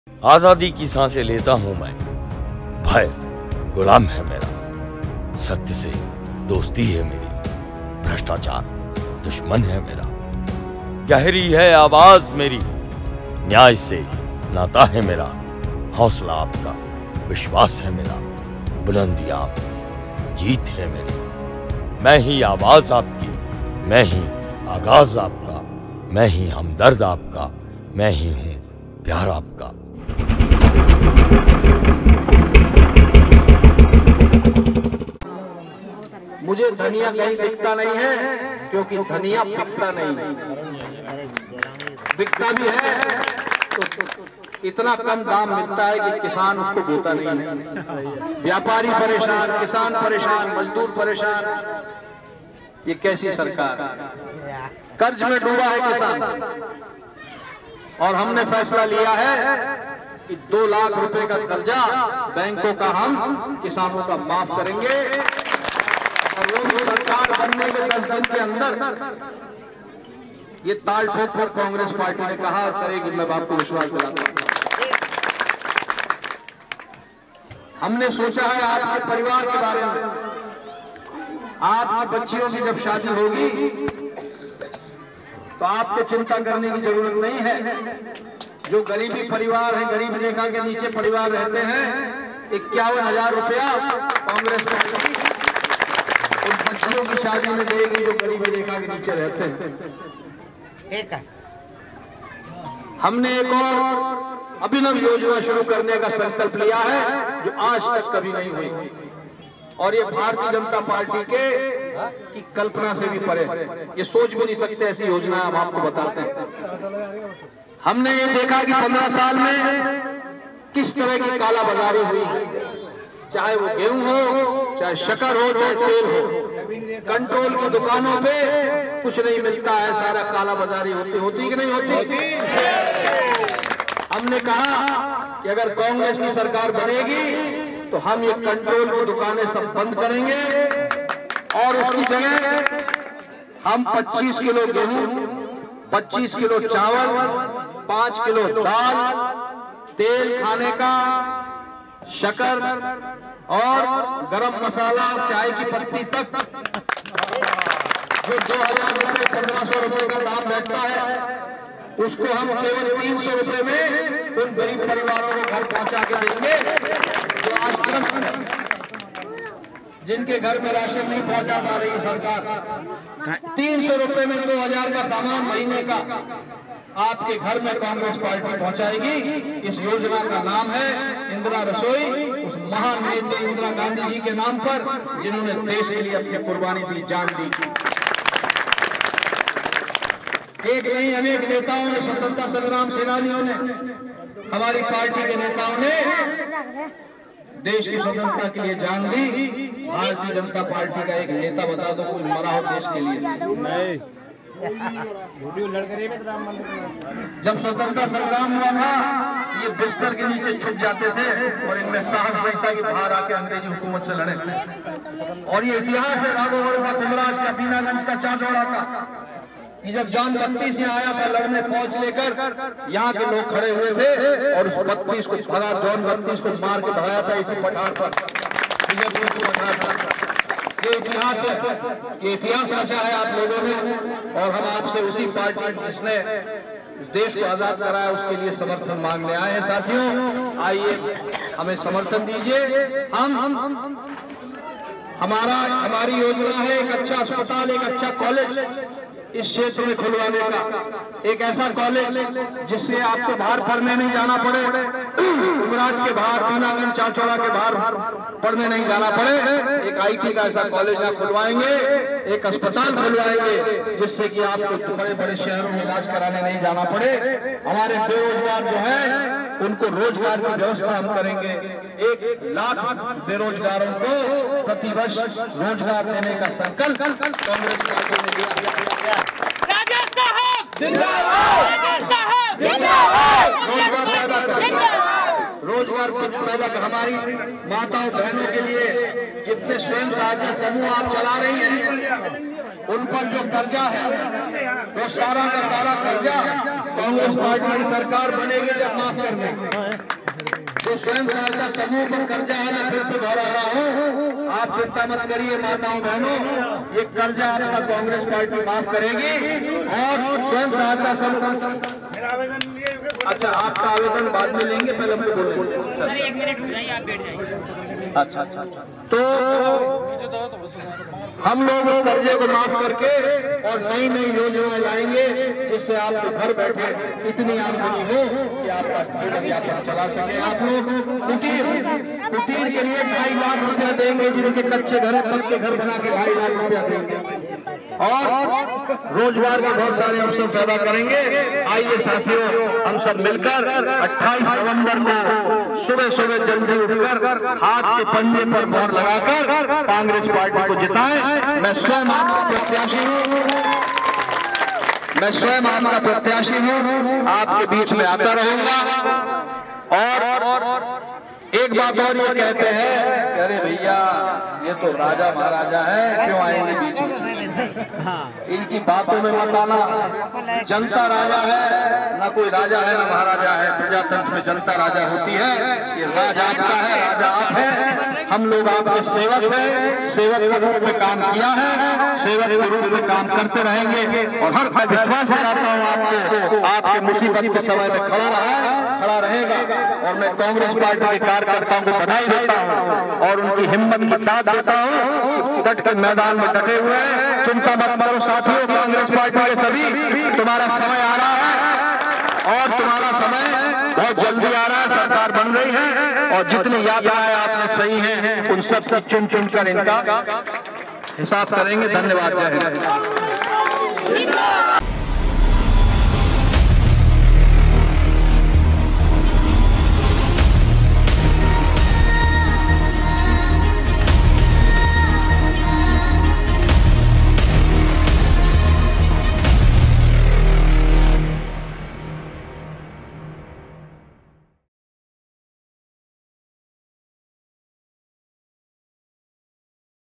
लक्ष्मण सिंह जी का कुम्भराज में जन संपर्क | Logical India
किसानो का क़र्ज़, ३०० रूपये में राशन, शिक्षा, रोज़गार, आदि मुद्दों पर लक्ष्मण singh जी अपने विचार रखे!
अंत में उन्होंने कहा की में राजा नहीं सेवक हूँ